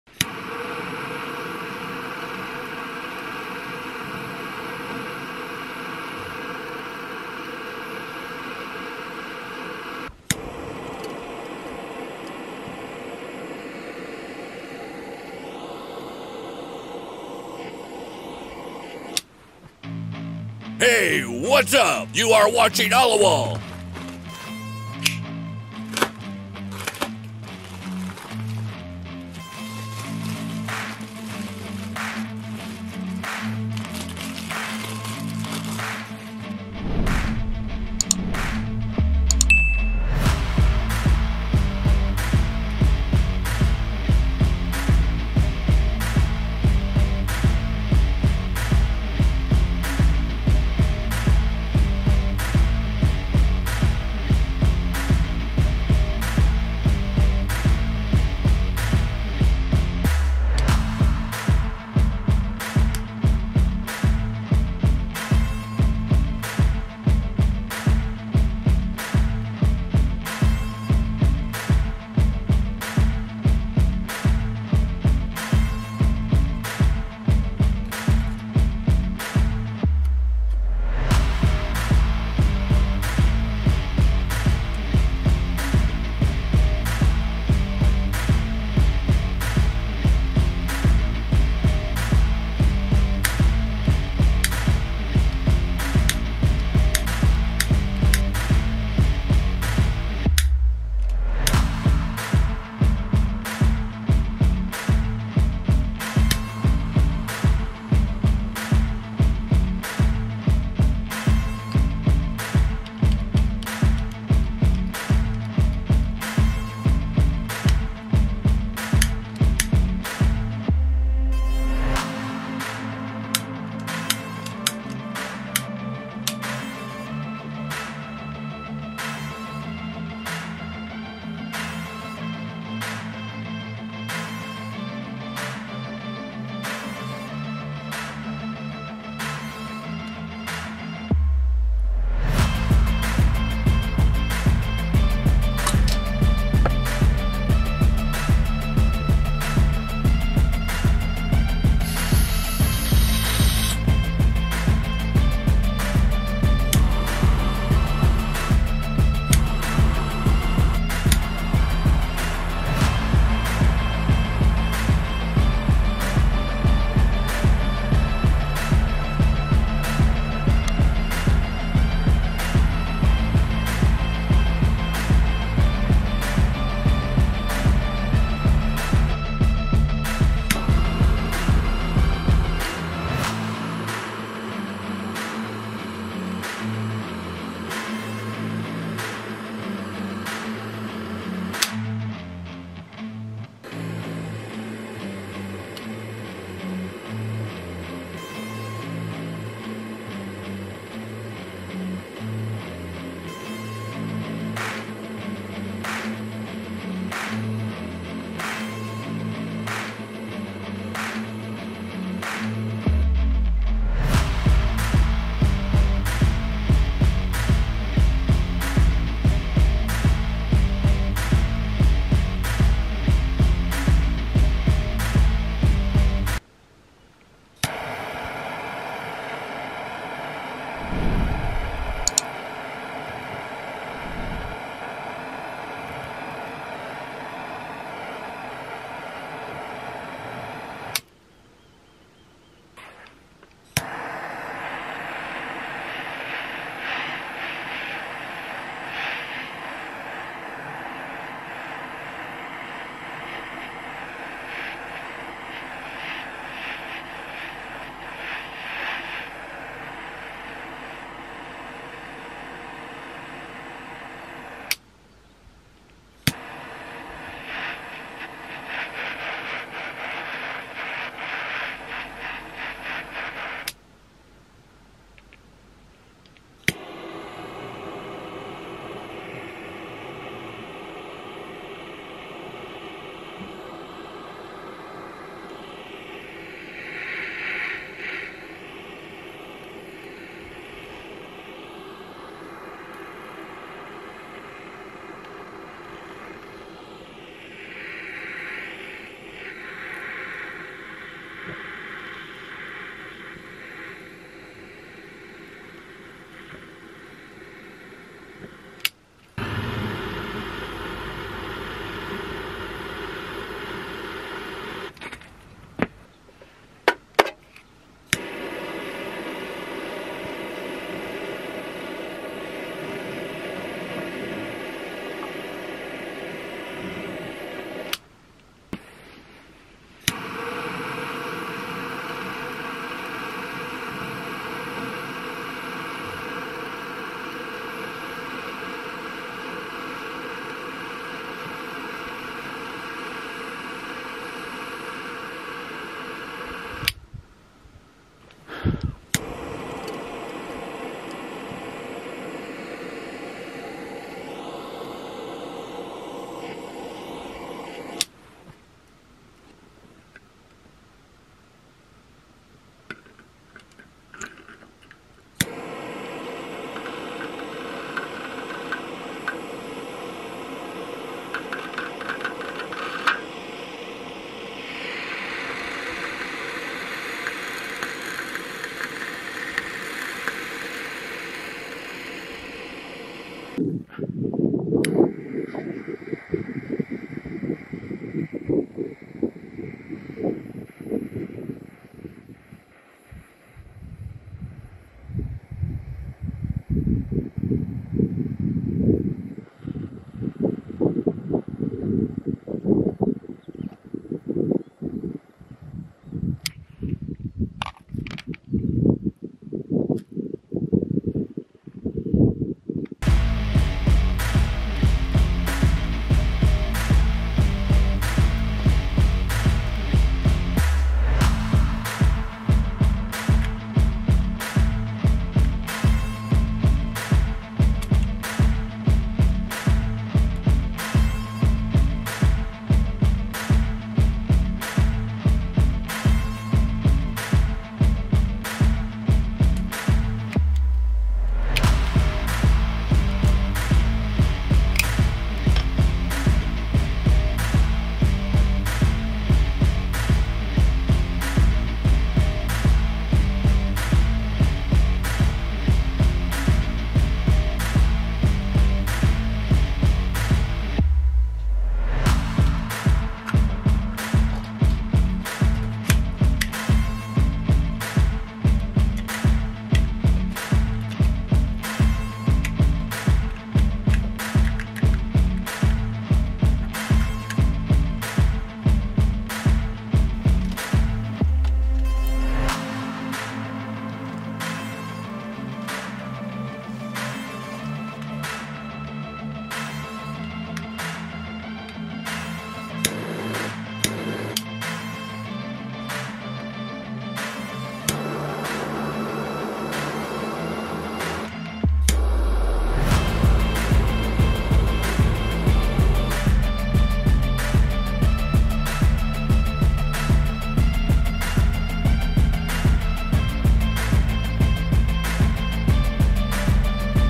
Most Powerful Torch Lighter Test + Refill
0:00 - Intro
0:24 - Unboxing Torch Lighter
2:07 - Torch Lighter Refill
3:47 - Torch Lighter Test
6:53 - What's inside Torch Lighter

Aliexpress shop
Amazon shop

Turbo Metal Blue Flame Gas Lighter Spray Gun Kitchen Cooking Smoking Accessories Windproof BBQ Jewelry Welding Cigar Lighters